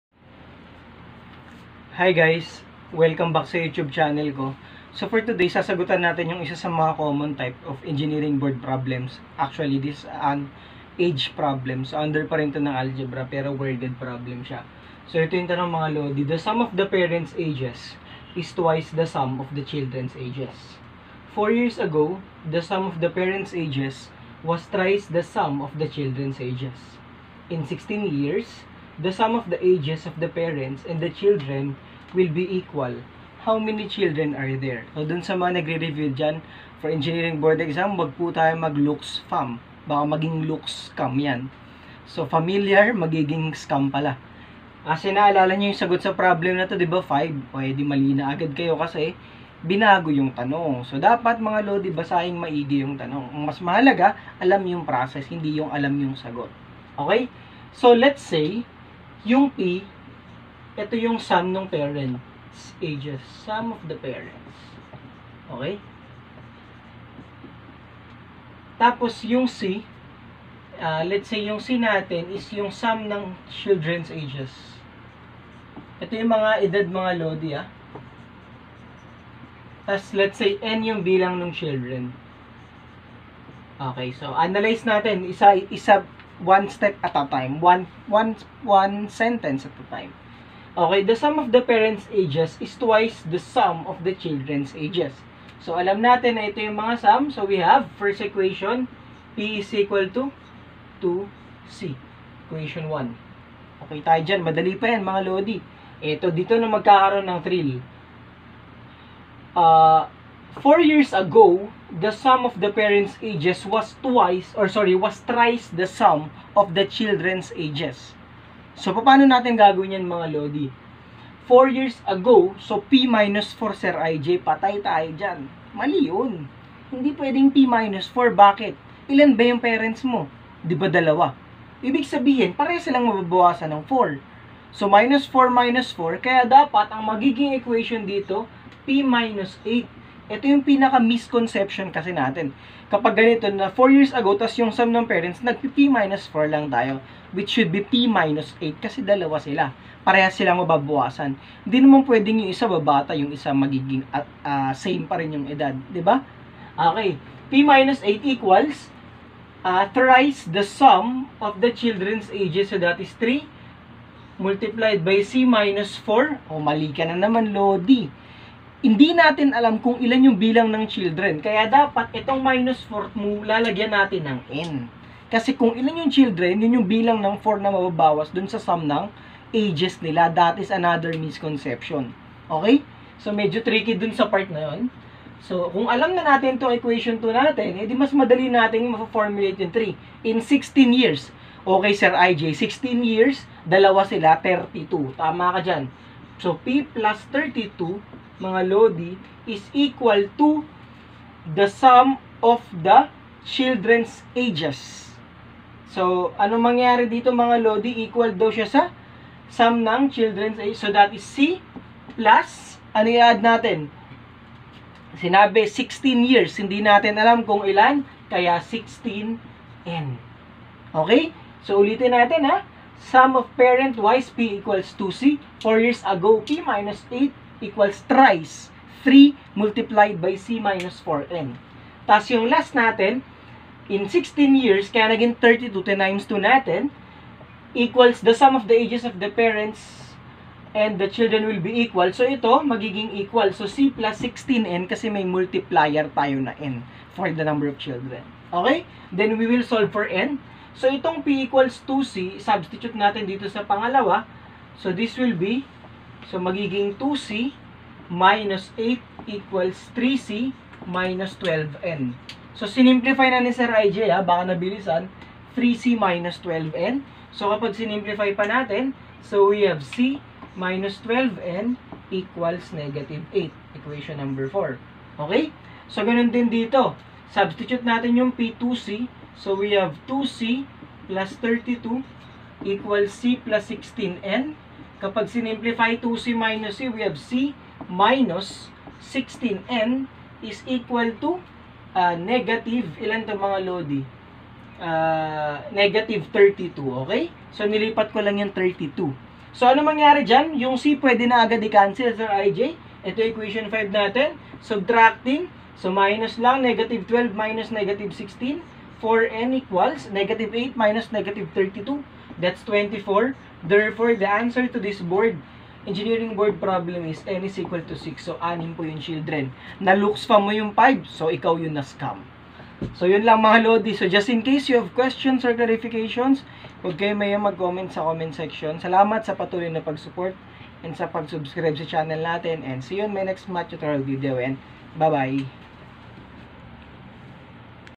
Random Math Problem: Algebra
The sum of the parents' ages is twice the sum of the children's ages. Four years ago, the sum of the parents' ages was thrice the sum of the children's ages. In sixteen years, the sum of the ages of the parents' and the children will be equal. How many children are there?